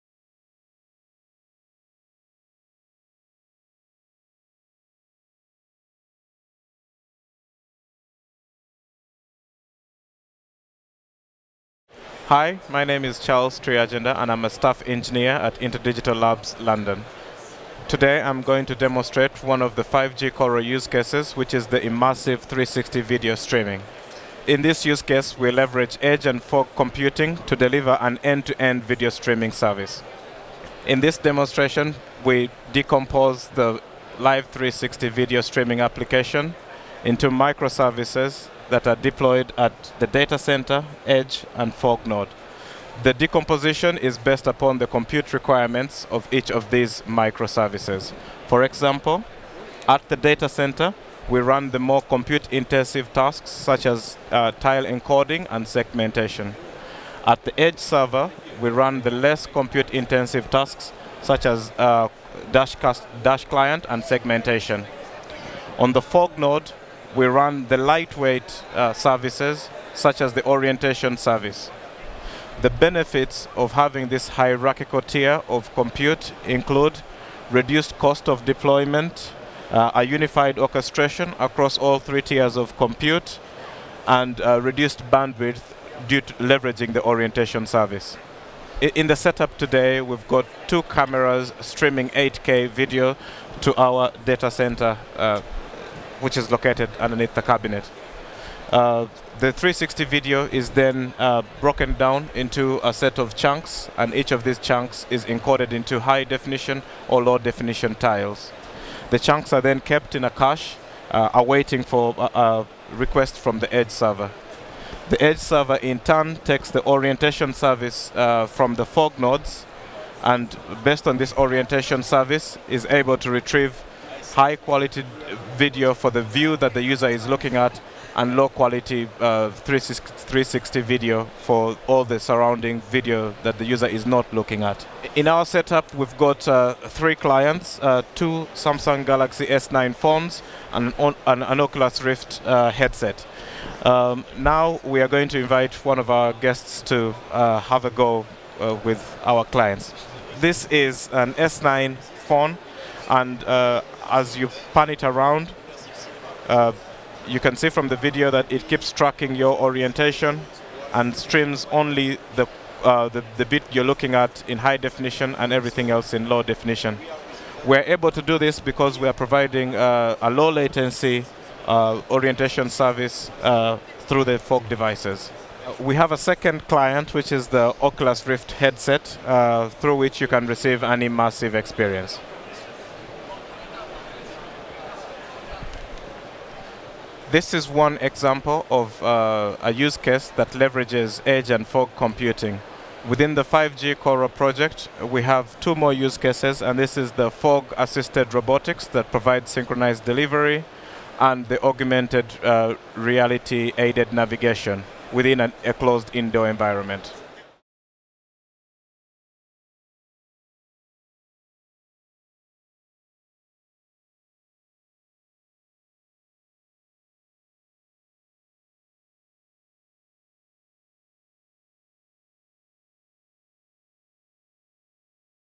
5G-CORAL Demo (Edge Computing)
- This demonstration hosted by Charles Turyagyenda, Staff Engineer at InterDigital, highlights the benefits of micro-services based distributed computing at the extreme edge of the 5G network. Experience E2E 360 video streaming deployed cost-efficiently across three tiers of computing nodes (low, medium and high end) all under unified orchestration and control.  The demo features two 360-degree cameras, each capturing a separate event happening in a different area of the booth, and users can view video streaming from either camera location, using a smartphone or oculus rift goggles while consuming only a fraction of the bandwidth.